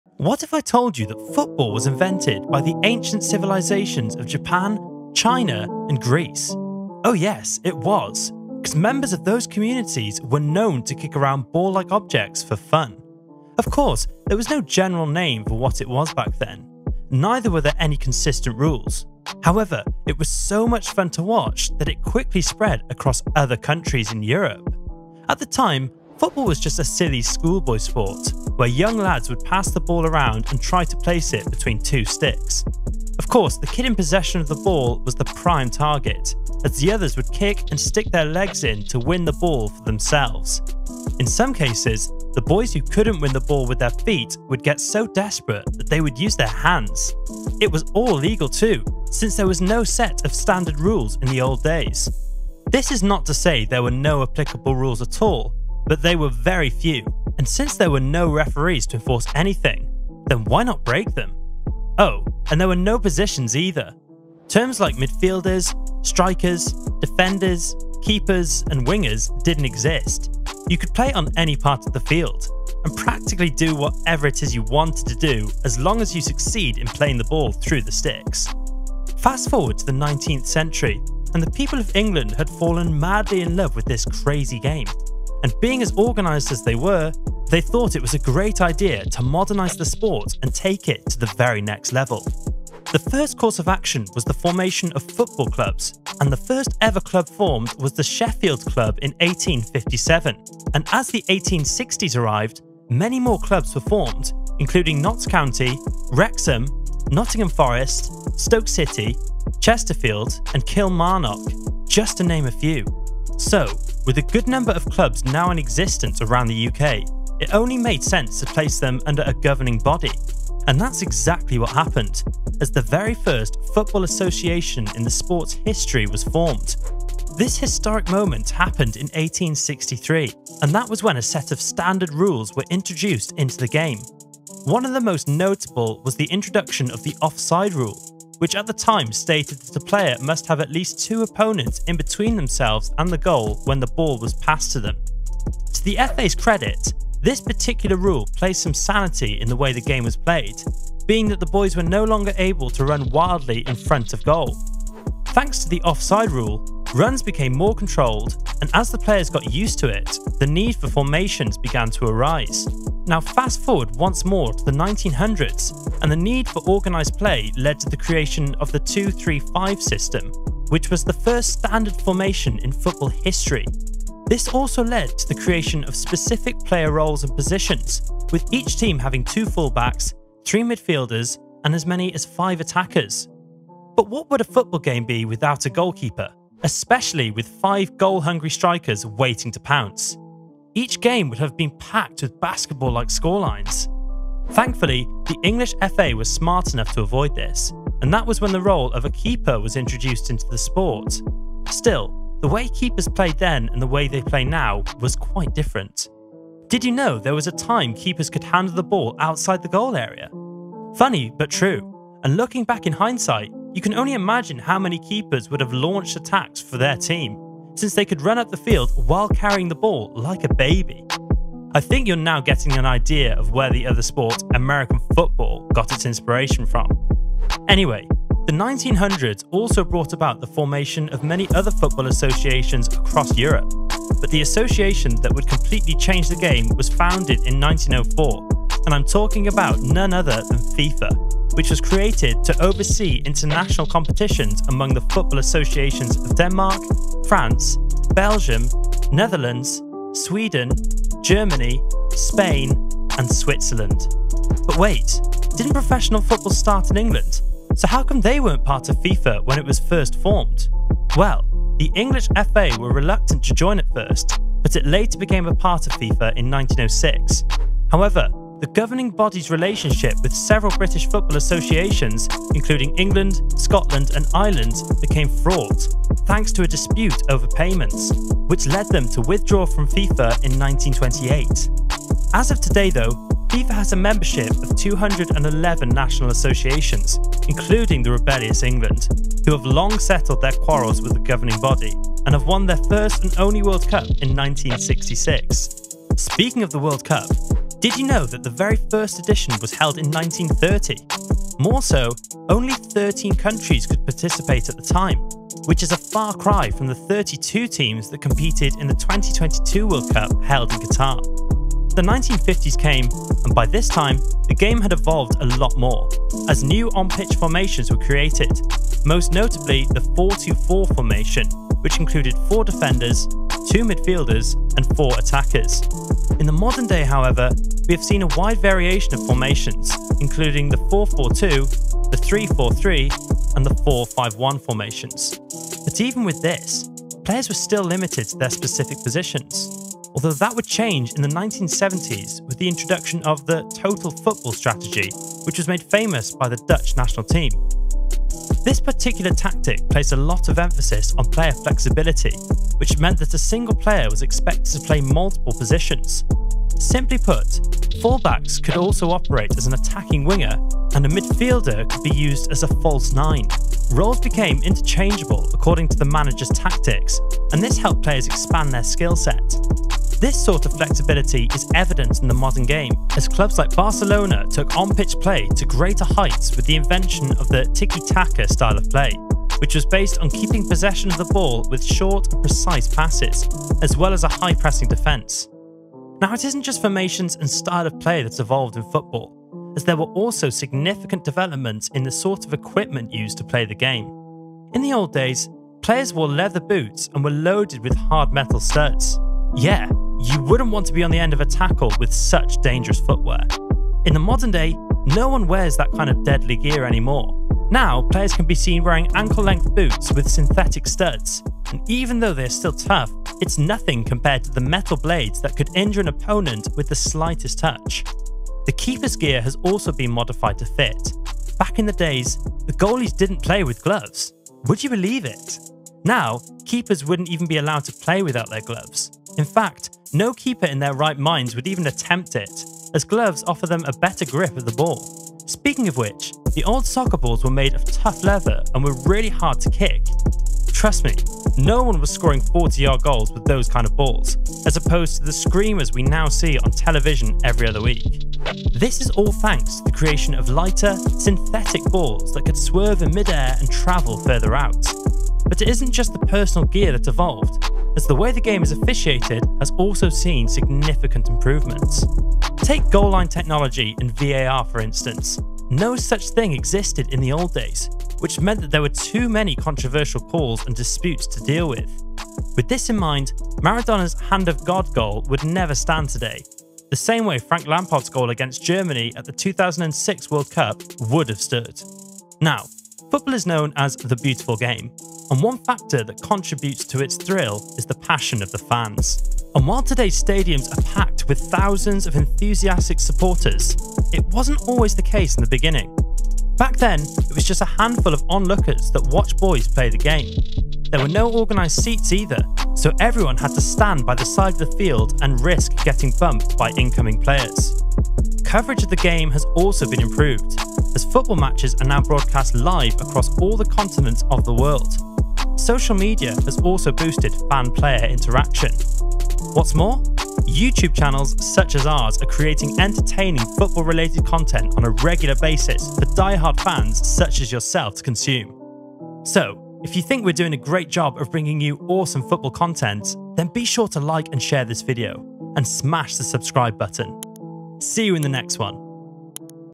The Evolution Of Football: Then vs Now
What if I told you that football was invented by the ancient civilizations of Japan, China, and Greece? Oh, yes, it was; coz members of those communities were known to kick around ball-like objects for fun.